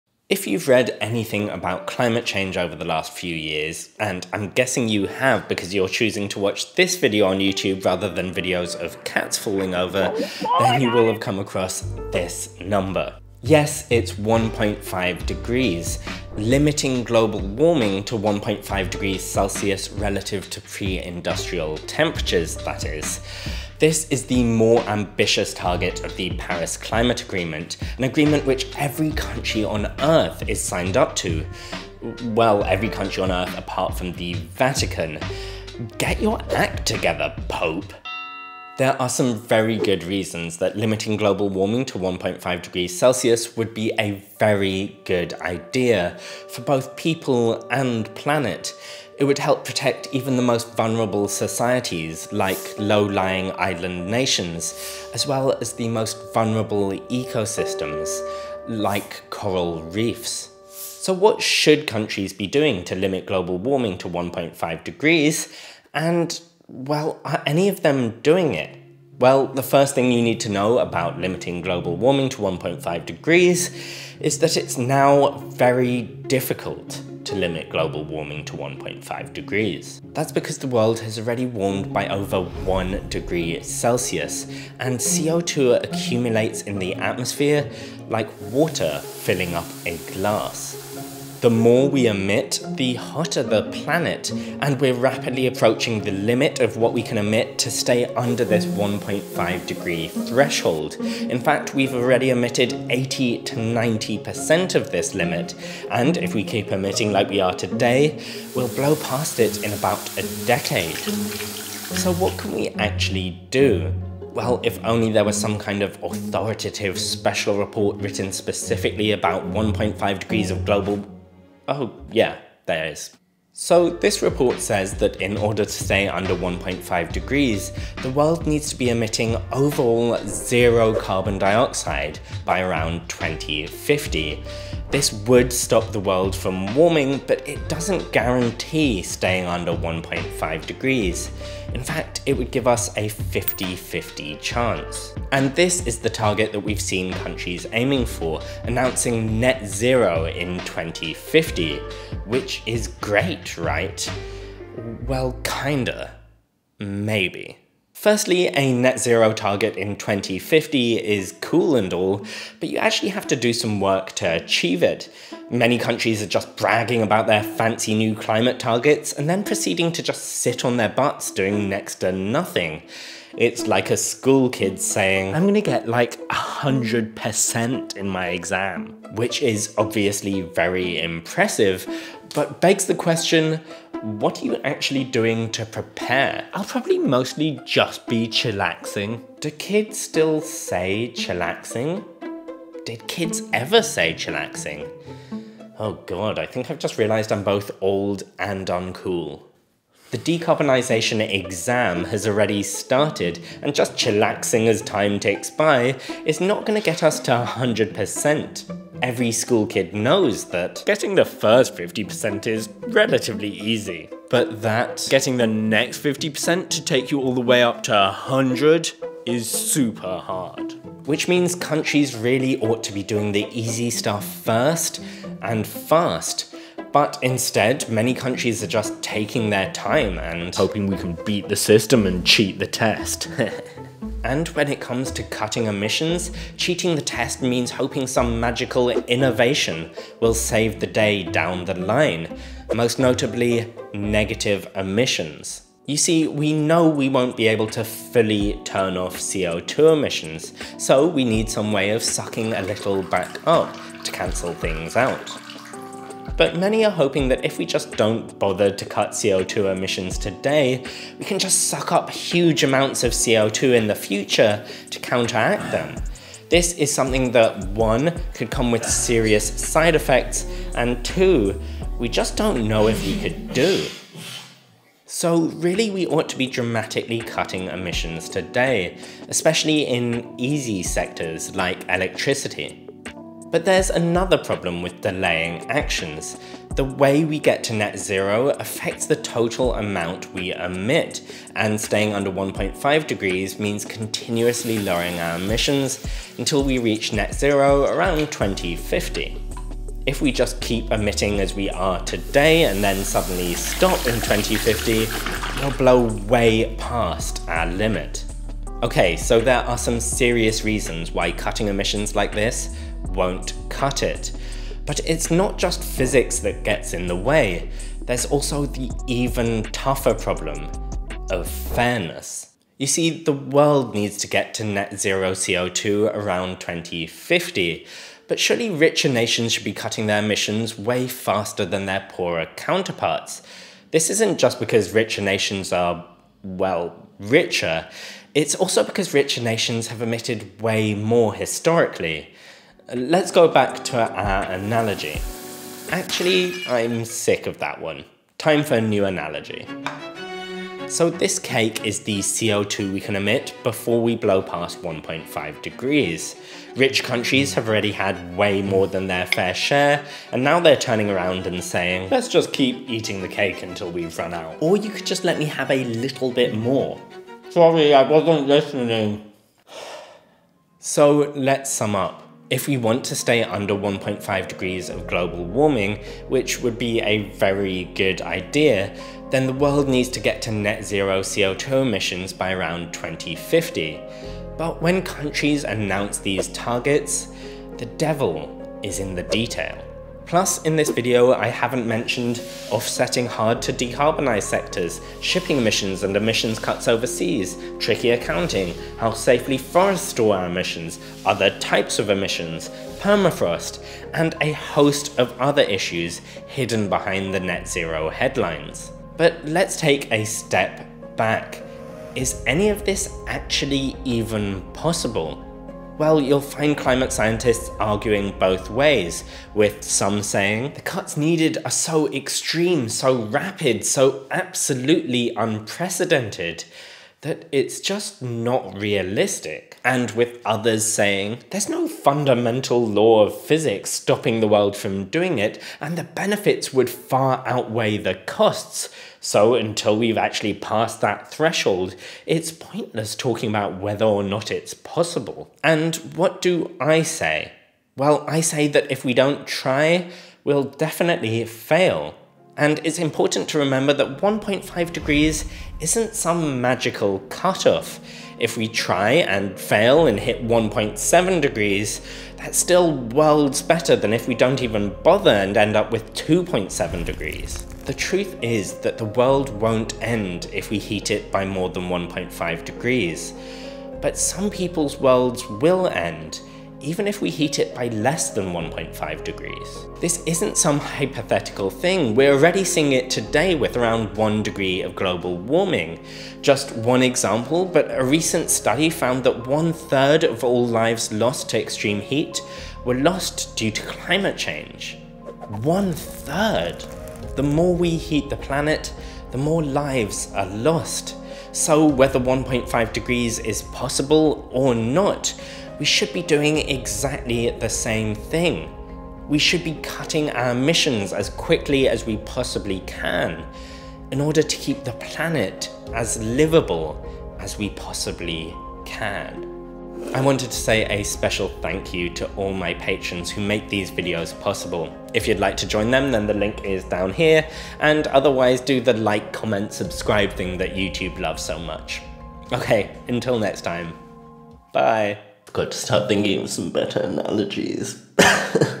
1.5 degrees: Is Net Zero 2050 enough?
What do we have to do to limit global warming to 1.5 degrees? And... is anybody doing it? Many countries have pledged net zero emissions by 2050, which may seem exactly what we need to halt climate change. But peek behind the headlines and you'll see a host of reasons the numbers don't add up.

==MORE INFO==

==THANKS==

Thanks to Joeri Rogelj for his input
Thanks to Daniel for his shirt and tie

cat video by Damsel Indetech
G7 footage by Canada Hub